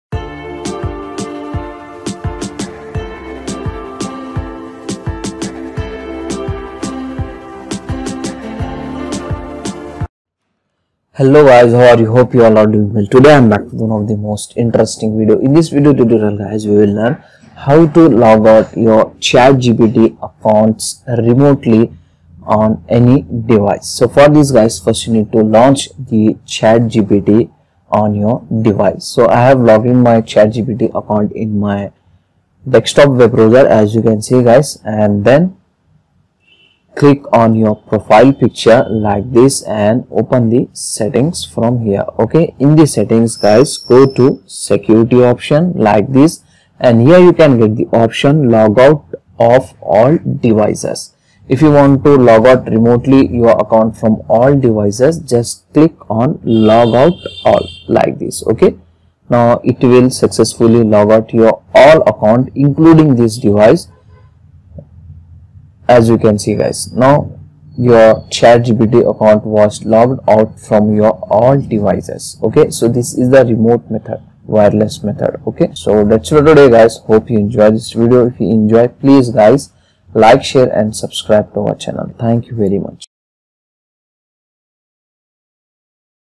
How to Remotely Logout Your ChatGPT Account from All Devices
How to Remotely Log Out of Your ChatGPT Account on All Devices

In this video, learn how to securely log out of your ChatGPT account from all devices remotely. Whether you've lost access to a device or want to ensure your account's security, this tutorial will guide you through the process step-by-step. Discover effective methods to manage your account's active sessions and safeguard your privacy. Watch now to take control of your ChatGPT account.